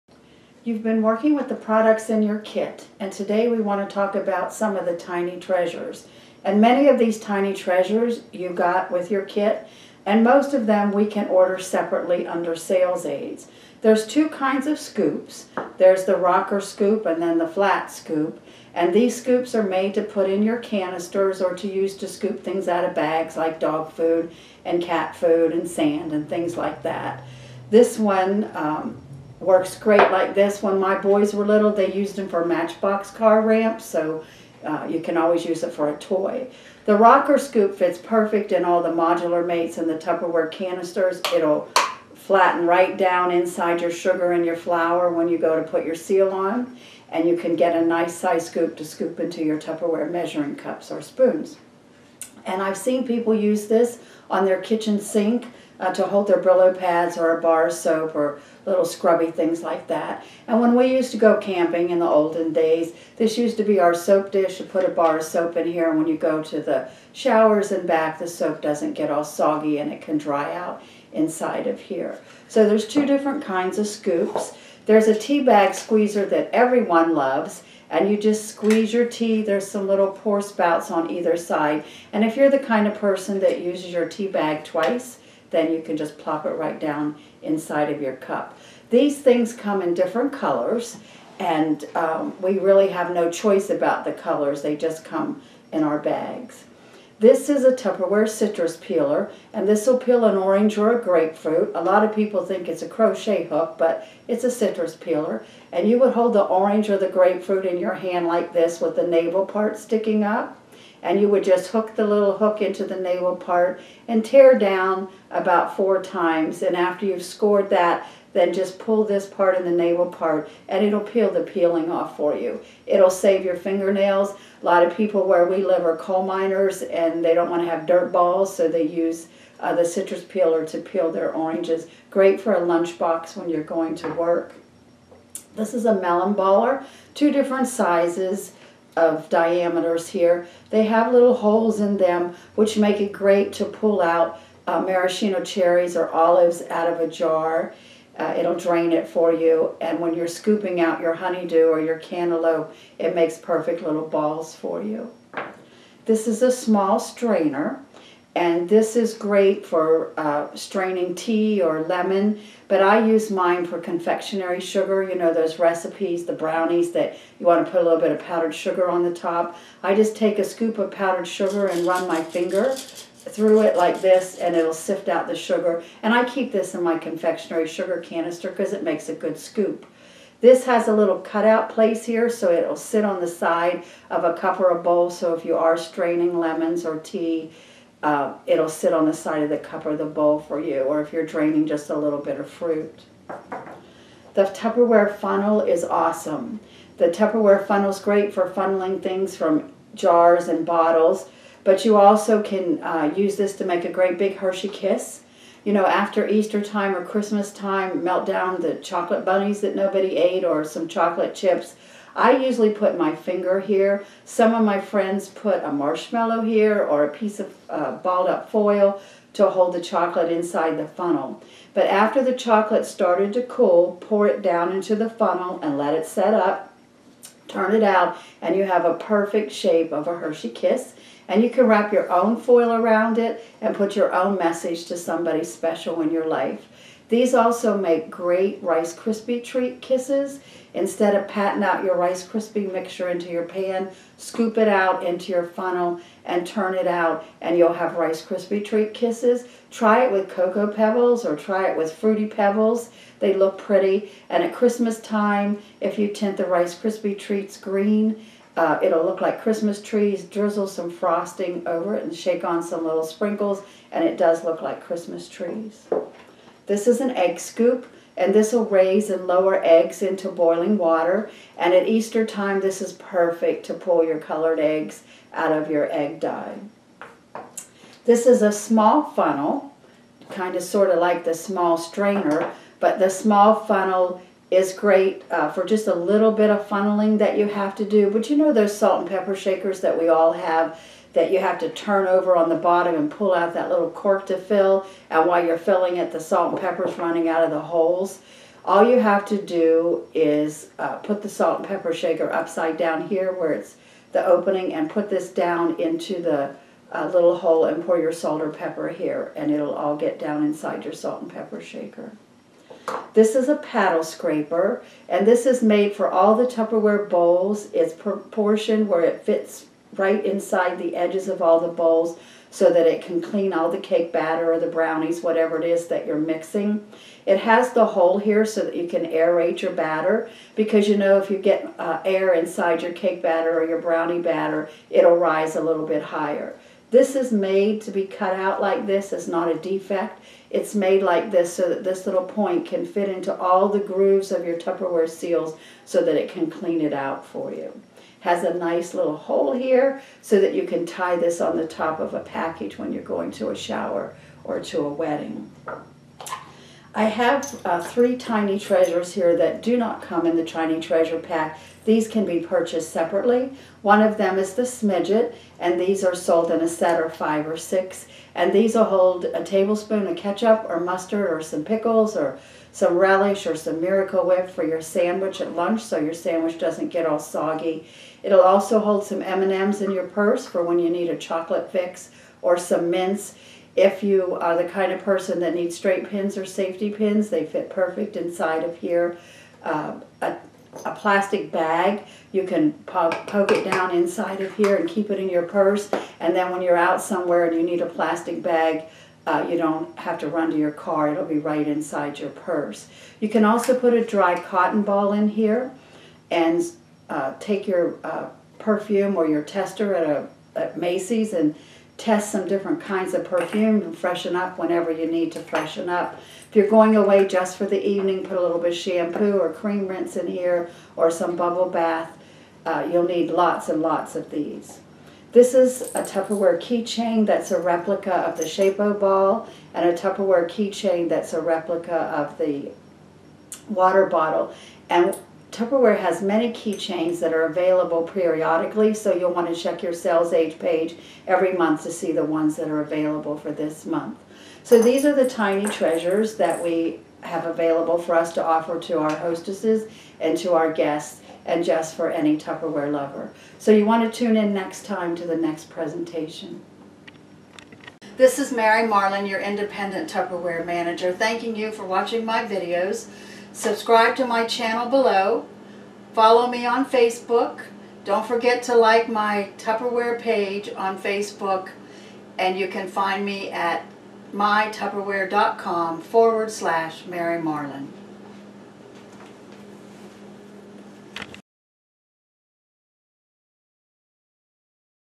Tupperware with Mary Marlin - Tiny Treasures
Showing off the Tupperware Tiny Treasures.

Like me on Facebook by searching: Tupperware by Mary Marlin, Independent Manager